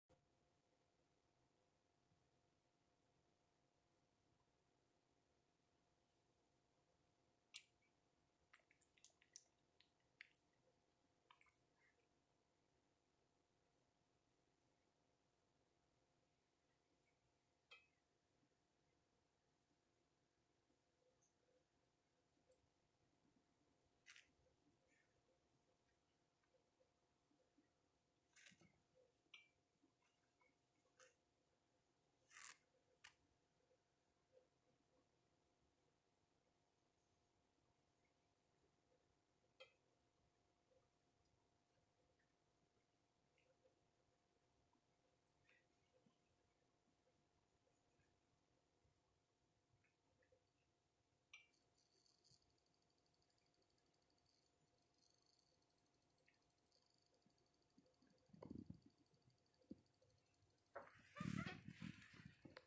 Head check by Penny Rose
Penny Rose is given her pet human a quick head check.. Look at her precious face as she carefully sorts through each hair looking for dead ends she needs to snap off.. Her little hands amaze me and her touches melts my heart.. She owns me.. Life is too short to fit in!!